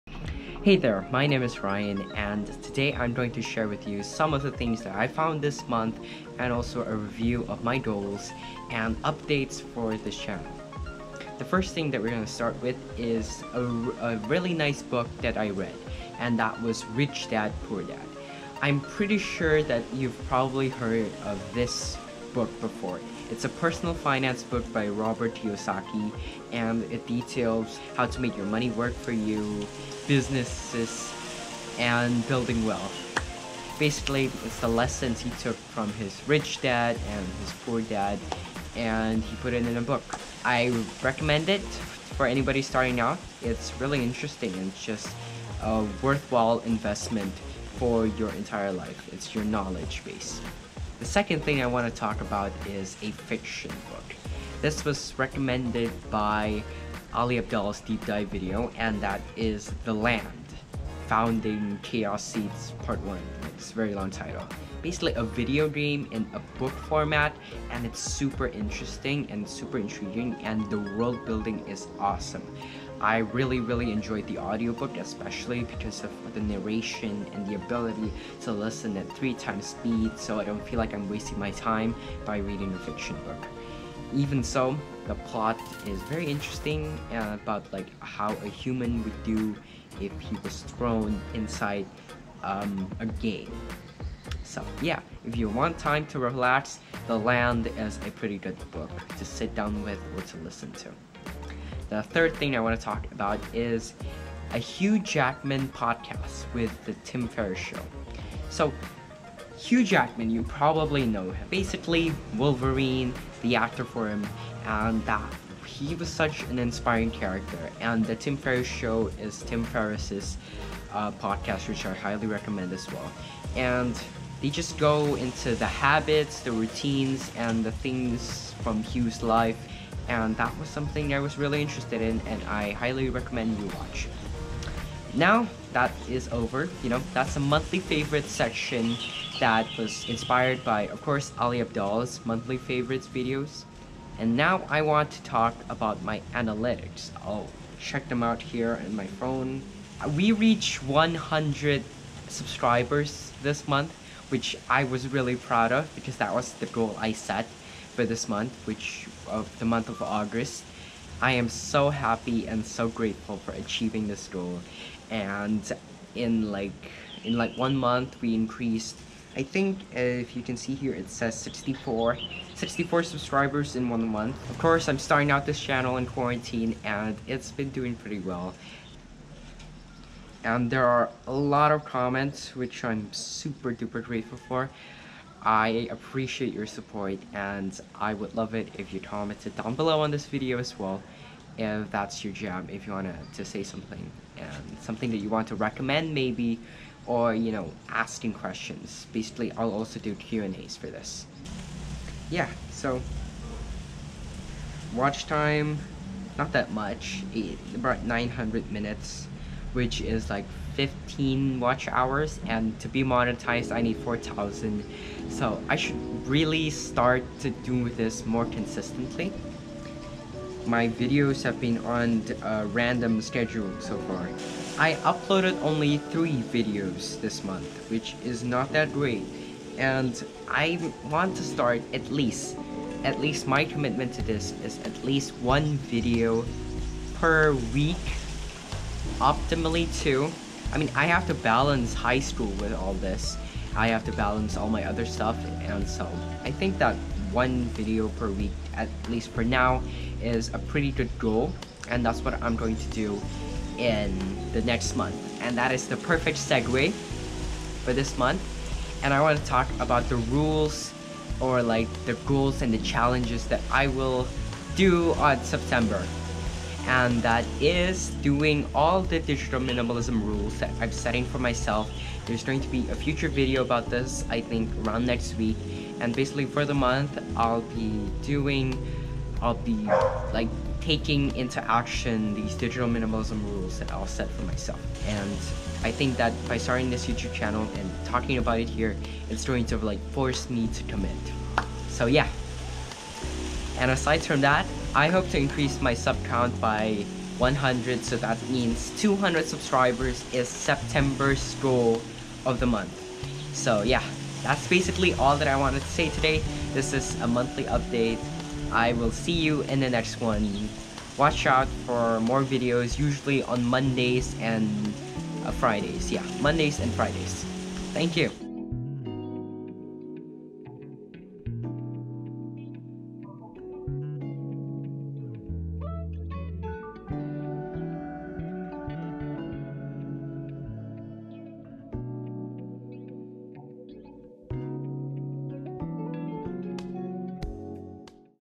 Rich Dad Poor Dad + The Land + Analytics // Monthly Review #1
Today we talk about books, a podcast, as well as future habits, updates and some analytics (reached 100 subscribers!)

Monthly Favorites were inspired by Ali Abdaal's monthly videos of course. I do a brief review of Rich Dad Poor Dad, The Land, the Hugh Jackman episode from the Tim Ferriss Show.

QUOTE TO PONDER //
People are frugal in guarding their personal property; but as soon as it comes to squandering time they are most wasteful of the one thing in which it is right to be stingy. - SENECA